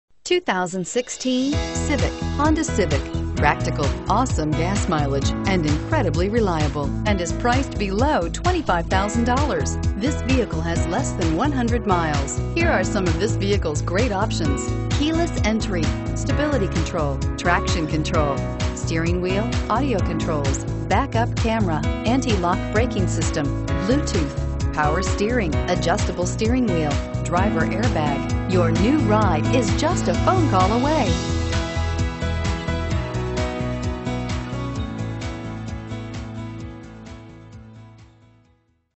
2016 Honda Civic Sedan Madison NJ 40206S
2016 Honda Civic Sedan LX

Madison Honda
280 Main Street

You'll love this 2016 Honda Civic Sedan. This is a 4dr car you'll want to take home. With 44 miles, it features Automatic transmission and an exterior color of Crystal Black Pearl. Call and get in touch with Madison Honda directly, and be the first to open the 4dr car door today!